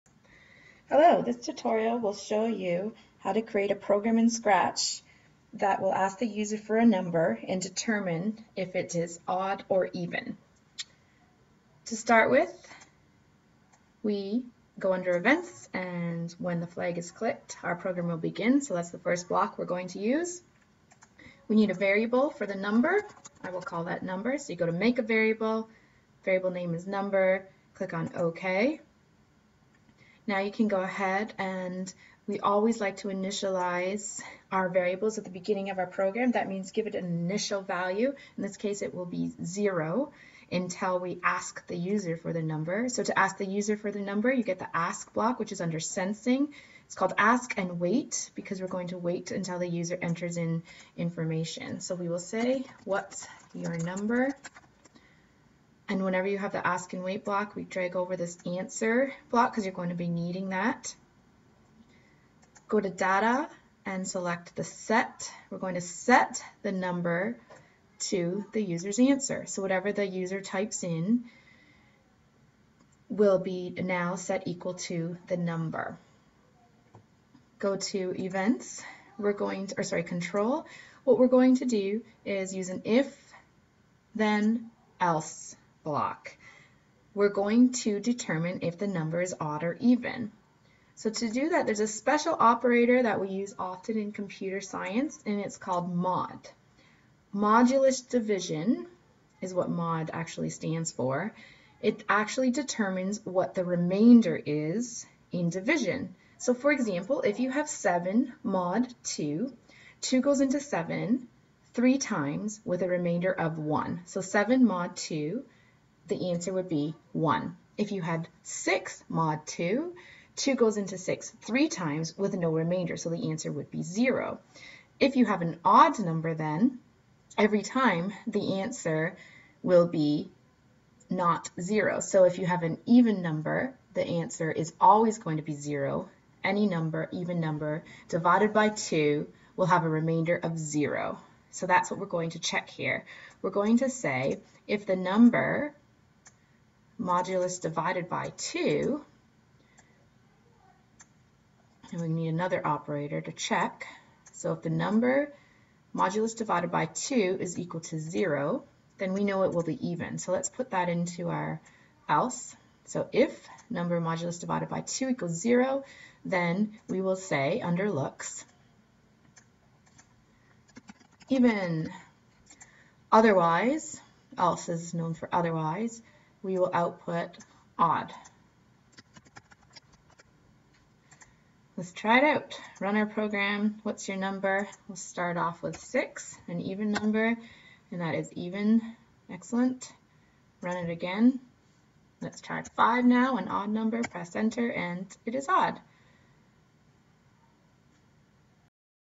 Odd or Even Scratch Tutorial
This tutorial will demonstrate how to write a program in Scratch that will determine if the number entered by the user is odd or even.